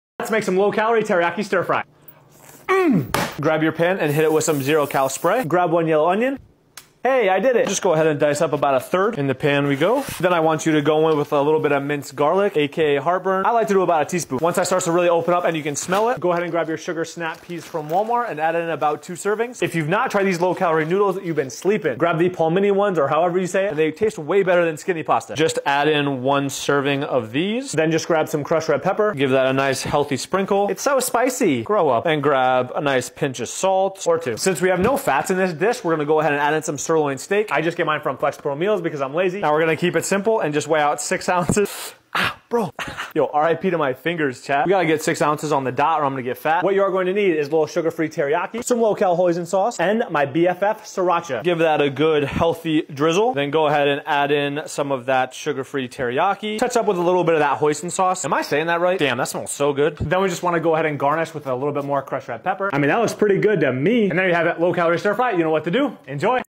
LOW CALORIE DINNER TO GET SHREDDED
Low-calorie teriyaki stir fry recipe made easy by @aaron.entres